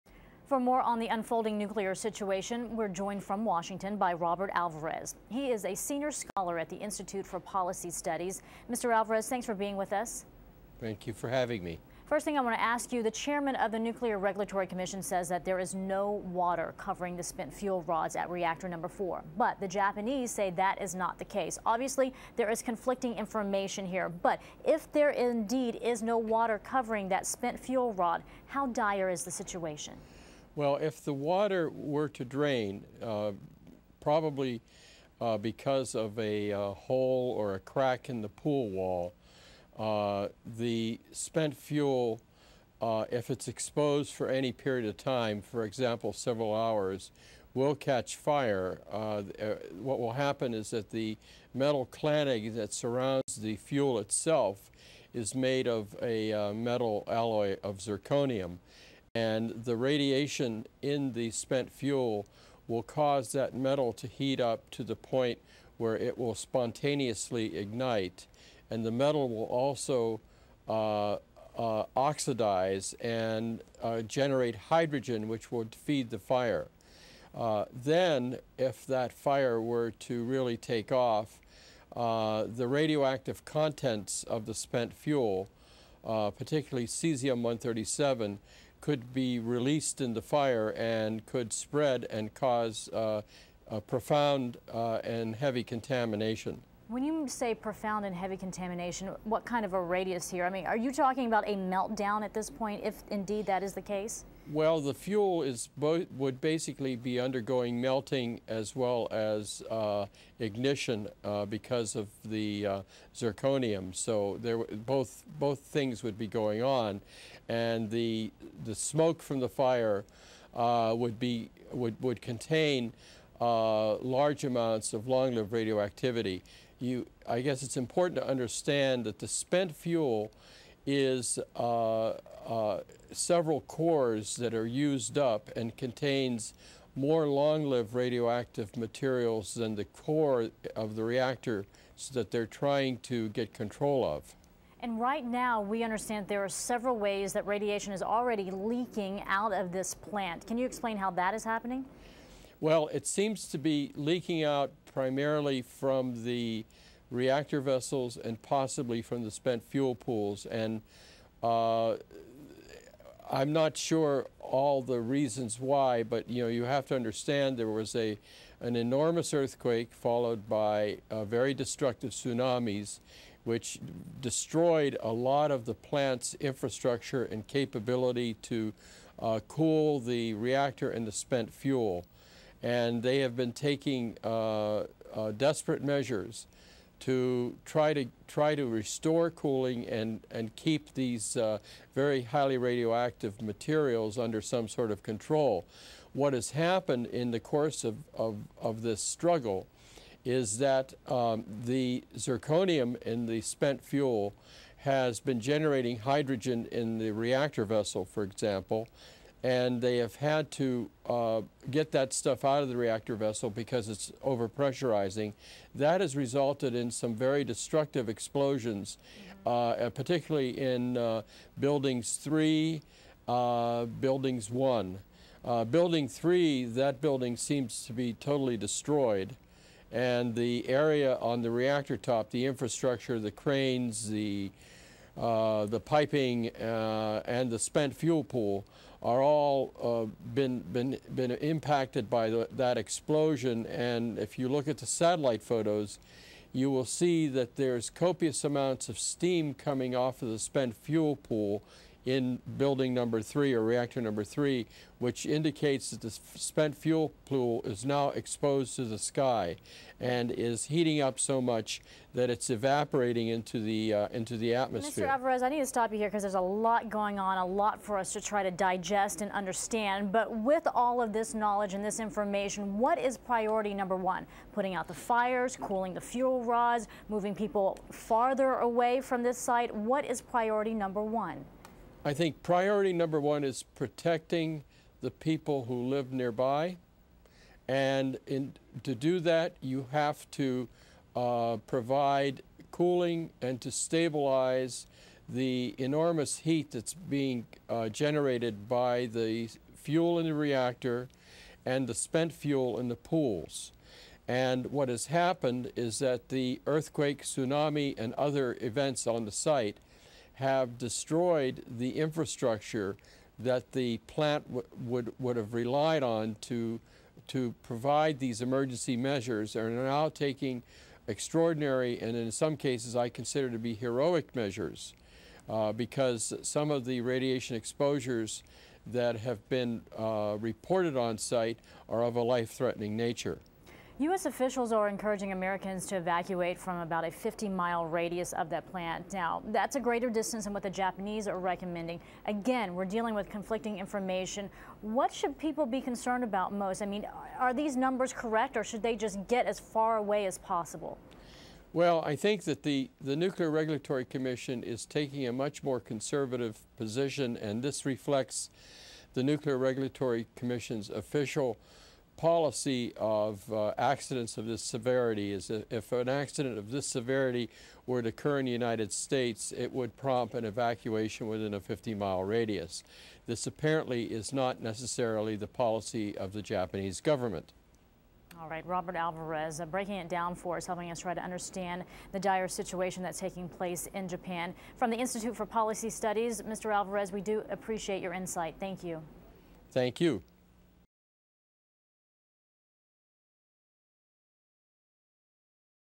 Japan's Nuclear Crisis
Robert Alvarez, Senior Scholar for the Institute for Policy Studies, discusses the latest on the nuclear crisis and how Japan is handling it.